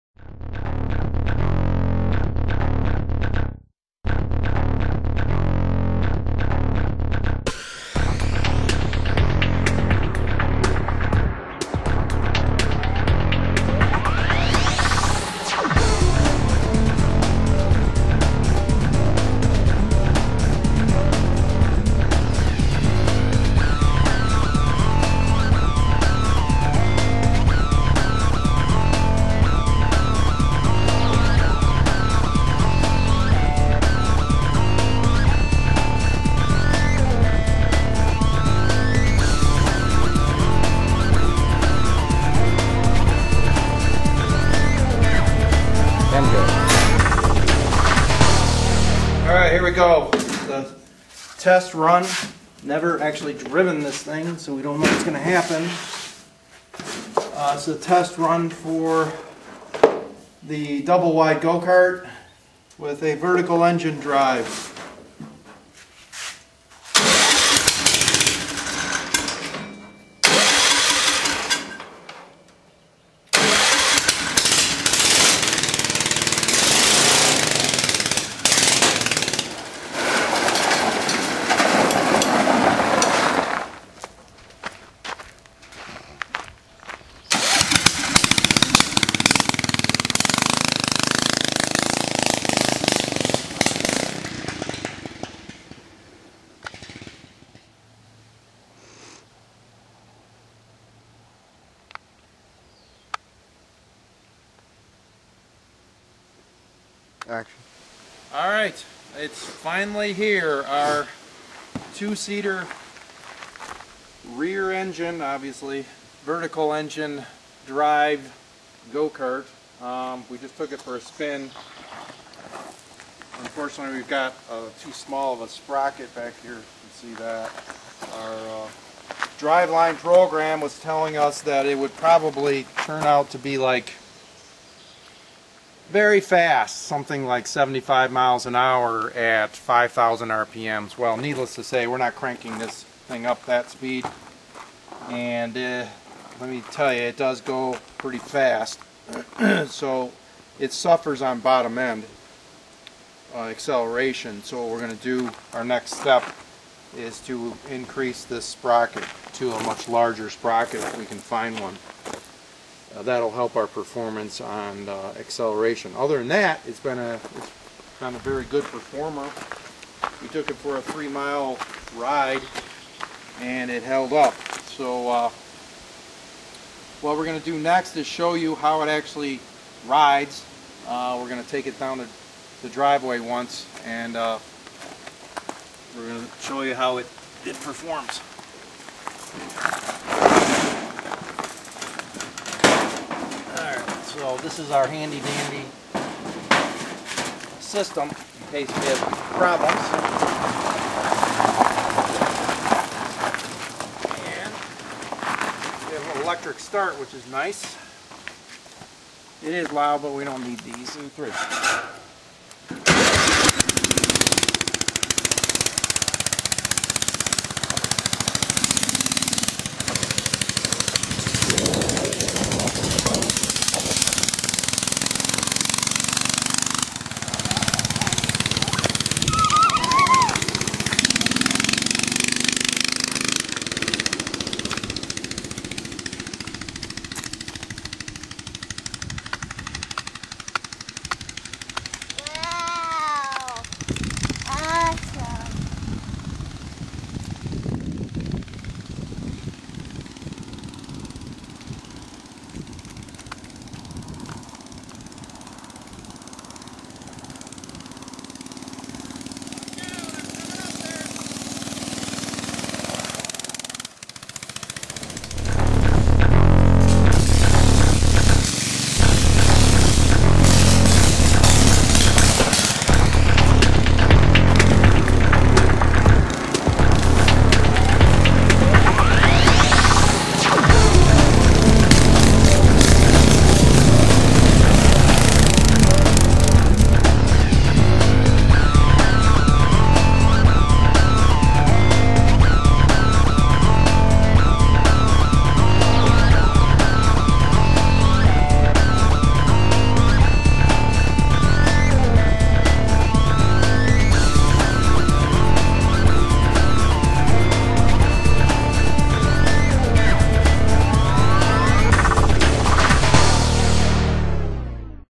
Vertical Engine GoKart: 2 Seater Phil Alpha 12 Go Kart
This go kart was started using a riding lawnmower components to make.  The engine, a 10 hp Techumseh, uses a vertical engine drive derived directly from the GoKartBuilding 202 course.  Subsequent driveline changes were accomplished using the GoKart Building 203 course that has a gokart drive line program that optimizes hill climbing and ultimate driving performance. Visit the GoKartGuru.com for more details.